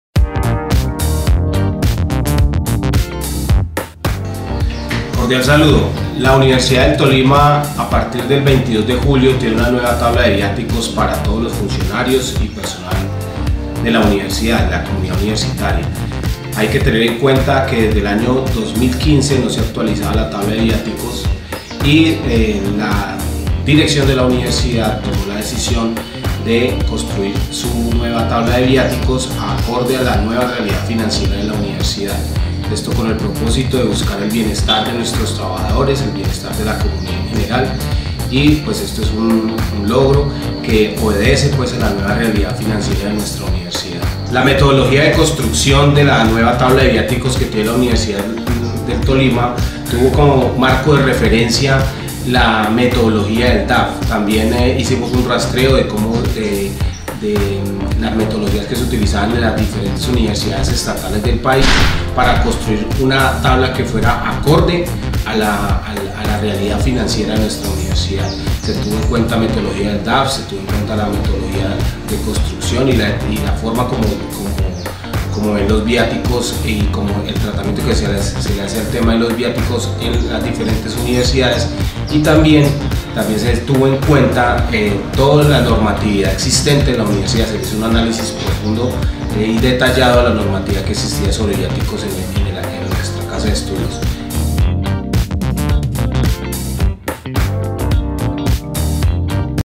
LaUTAvanza | Nueva tabla de viáticos
El Vicerrector Administrativo (e) habla sobre la nueva tabla de viáticos   para funcionarios de la Universidad del Tolima.